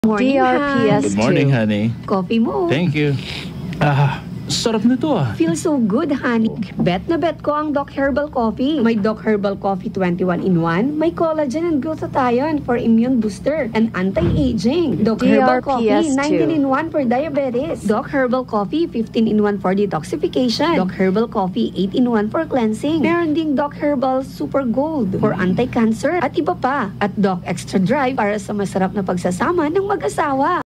Dok Herbal Coffee Line Radio Ad 2020-2024 31s
Ito po ang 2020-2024 Radio Ad ng Dok Herbal Coffee Line na gawa ng Yumnan Duk's Health Products. Mabibili lamang ito sa lahat ng mga suking botika sa Luzon. (Main Version)

DRPS Two 7529: Dok Herbal Coffee Line Radio Ad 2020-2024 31s